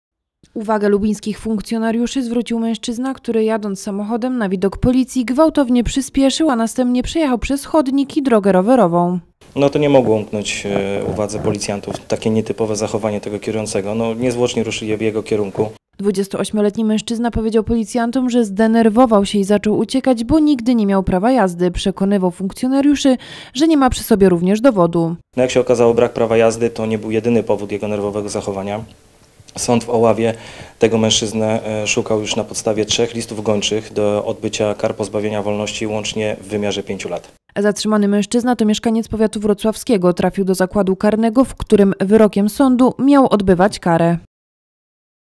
LUBIN. Spanikował na widok policji
Widząc patrol policji, gwałtownie przyśpieszył i zboczył z trasy. Przejechał przez tereny zielone, chodnik i drogę rowerową. Policjanci zatrzymali 280-letniego mężczyznę. Okazało się, że był poszukiwany trzema listami gończymi.